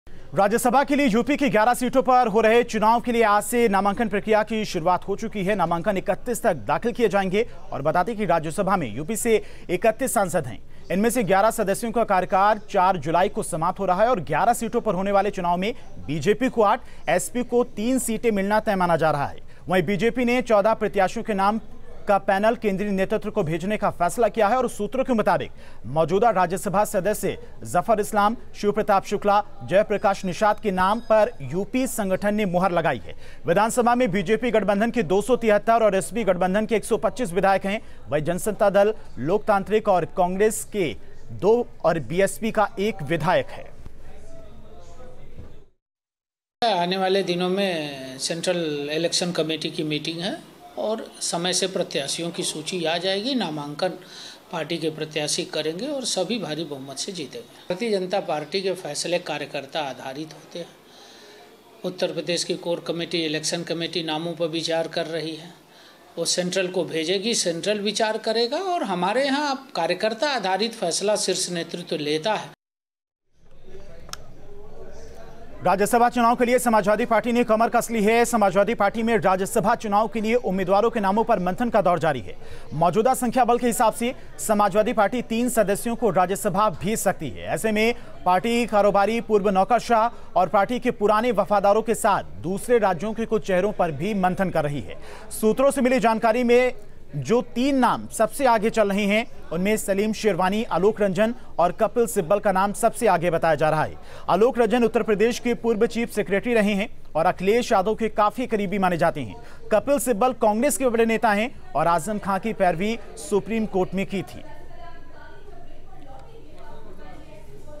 UP News : राज्यसभा की 11 सीटों पर होने वाले चुनाव में BJP-SP को मिलेंगी कितनी सीटें?, देखें वीडियो
Rajya Sabha Polls in Uttar Pradesh: उत्तर प्रदेश में राज्यसभा की 11 सीटों पर होने वाले चुनाव में बीजेपी को 8 और समाजवादी पार्टी को 3 सीटें मिलना तय है. विधानसभा में बीजेपी (BJP) गठबंधन के 273 और समाजवादी पार्टी (Samajwadi Party) गठबंधन के 125 विधायक हैं. राज्यसभा के लिए प्रदेश की 11 सीटों पर हो रहे चुनाव के लिए आज से अधिसूचना जारी होने के साथ ही नामांकन दाखिल करने का सिलसिला शुरू हो जाएगा. नामांकन 31 मई तक दाखिल किए जा सकेंगे.

Rajya Sabha Polls in Uttar Pradesh: In the elections to be held on 11 Rajya Sabha seats in Uttar Pradesh, BJP is sure to get 8 seats and Samajwadi Party 3 seats. The BJP alliance has 273 MLAs and the Samajwadi Party alliance has 125 MLAs in the assembly. With the issuance of notification for the Rajya Sabha elections to be held on 11 seats in the state from today, the process of filing nominations will start. Nominations can be filed till May 31.

News State Apps डाउनलोड करें:
Download Android and iOS Apps for more updates: